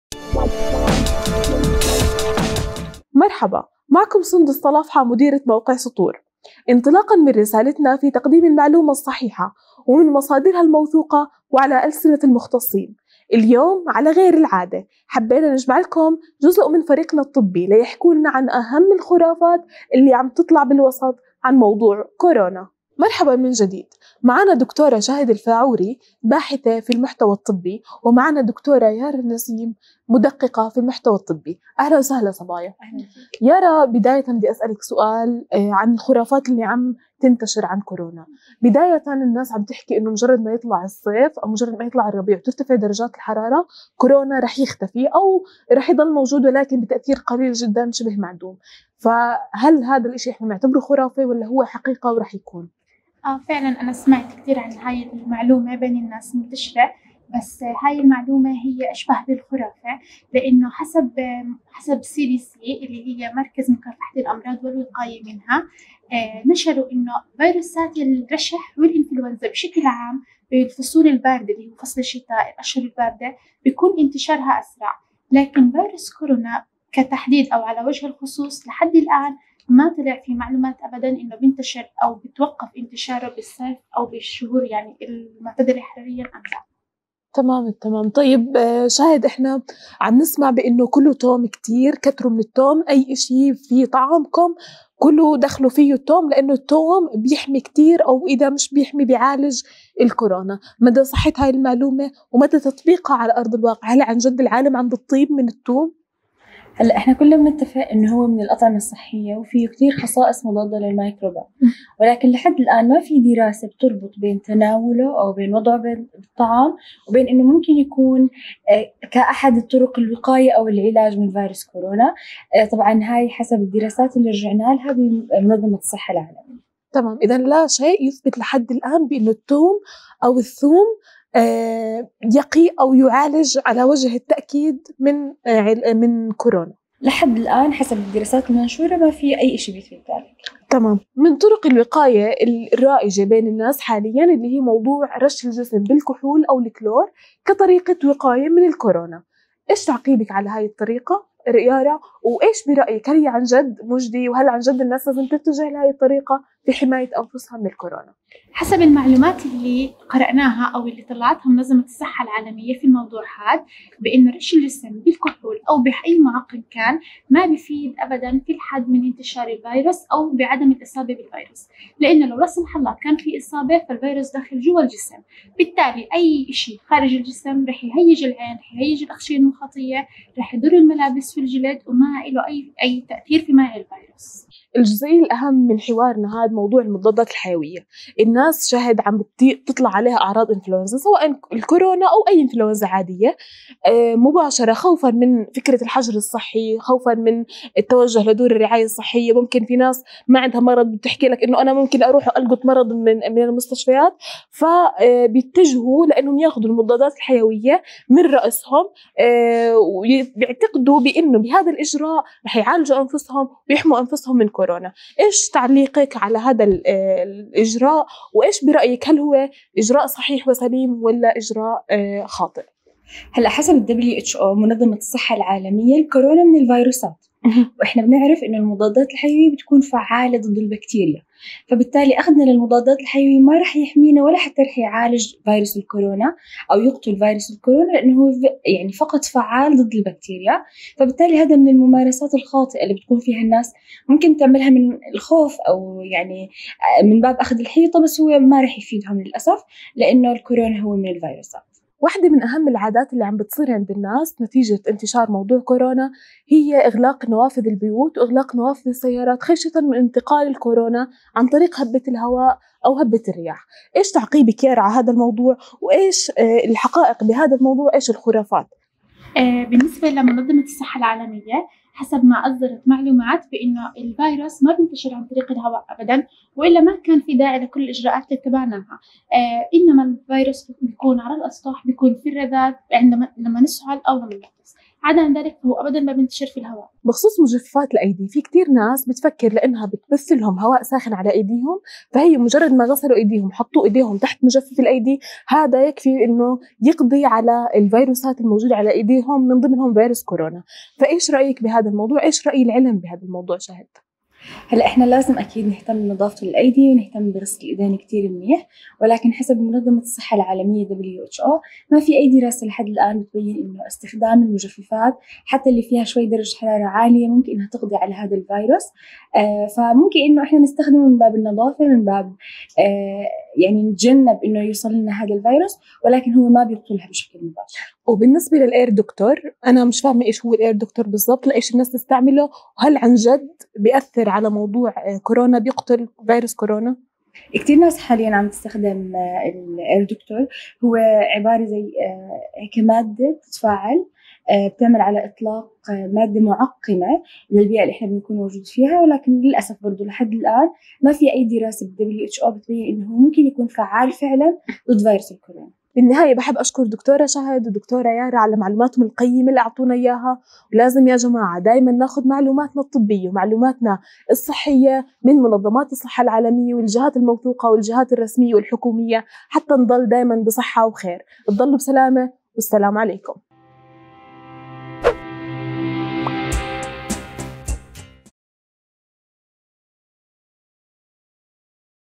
خرافات حول فيروس كورونا كوفيد 19
في هذا الفيديو تتحاور مديرة موقع سطور المهندسة سندس الطلافحة مع كل من الدكتورة الصيدلانية شهد الفاعوري باحثة في المحتوى الطبي في موقع سطور والدكتورة الصيدلانية يارا نسيم مدققة المحتوى الطبي في  موقع سطور حول الخرافات حول فيروس كورونا كوفيد 19، وتم التأكيد أنَّه لا يوج أي دراسة أثبتت أنَّ ارتفاع درجات الحرارة يمكن أن تحد من انتشار فيروس كورونا الجديد كوفيد 19، وتم التنويه إلى أنَّ الثوم لا يعتبر واقي من فيروس كورونا الجديد كوفيد 19 ولا يقوم بعلاجه، بالإضافة أنَّ المضادات الحيوية غير فعالة في علاج فيروس كورونا كوفيد 19.

سطور موسوعة عربيّة شاملة، تعالج الموضوعات التي تهمّ القارئ العربيّ، علميّةً كانت أم إنسانيّة، وتطرحها وفقَ رؤية موضوعيّة ومنهجيّة.